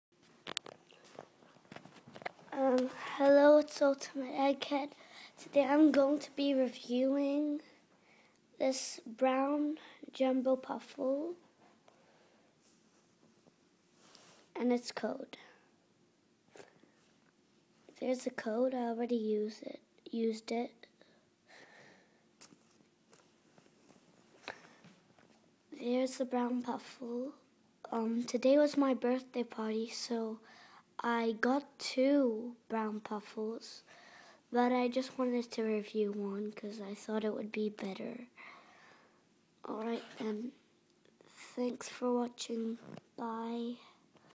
Jumbo brown puffle
This is a toy review of a jumbo brown puffle